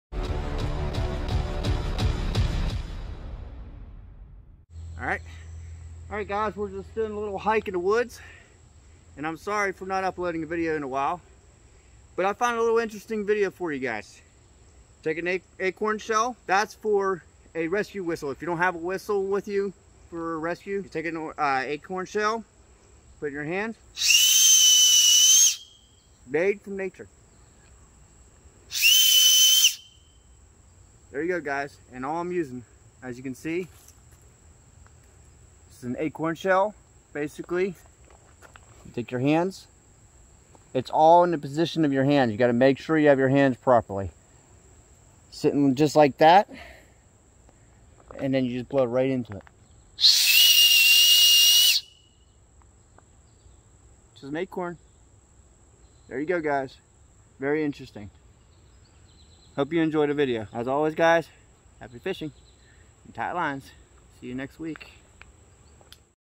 DIY How to make a Whistle from an Acorn
In this DIY video we are going to teach you how to use an acorn for a whistle.